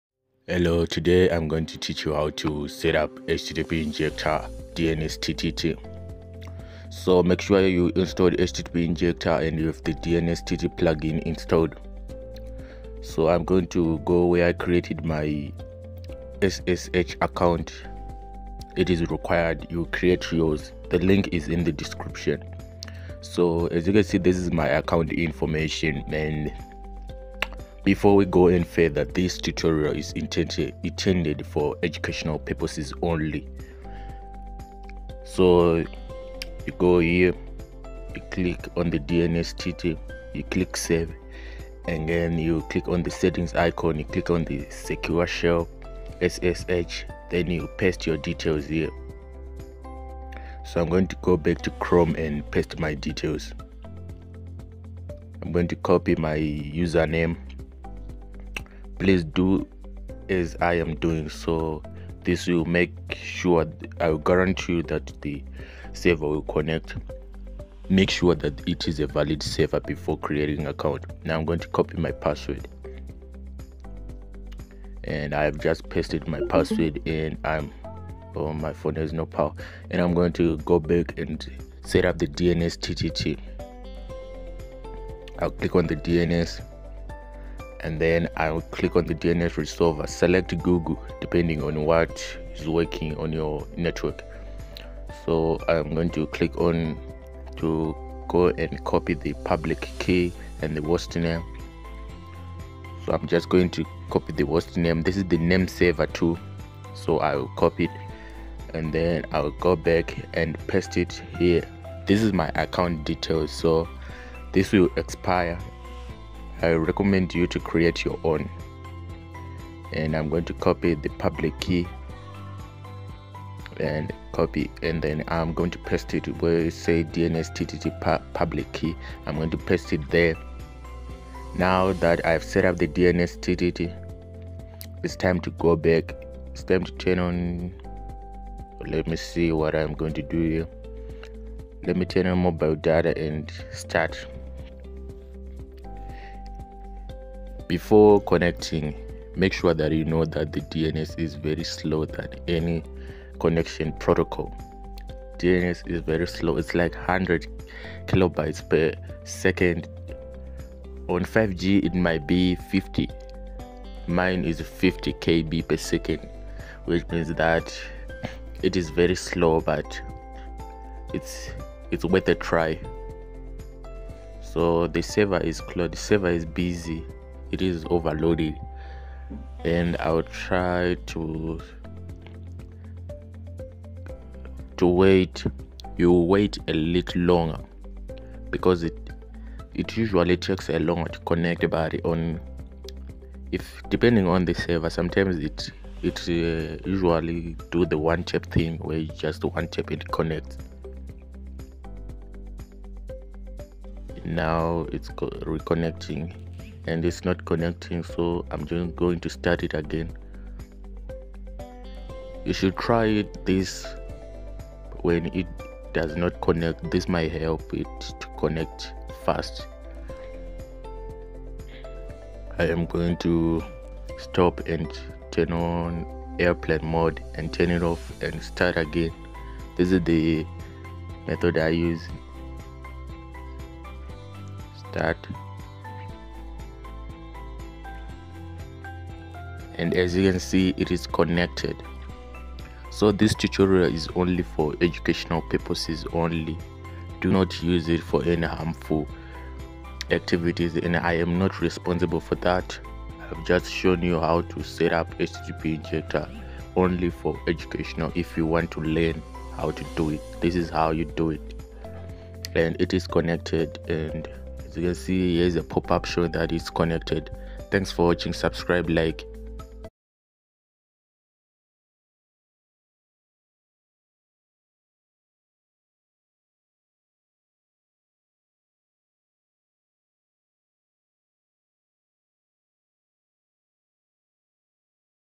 HTTP Injector DNSTT Setup Tutorial | Easy Step-by-Step Guide
Learn how to set up HTTP Injector DNSTT with our easy step-by-step tutorial! In this video, we'll show you how to configure HTTP Injector with DNSTT settings for fast and secure internet. Perfect for beginners, this guide will help you unlock the full potential of HTTP Injector. Follow along and boost your internet speed and security today!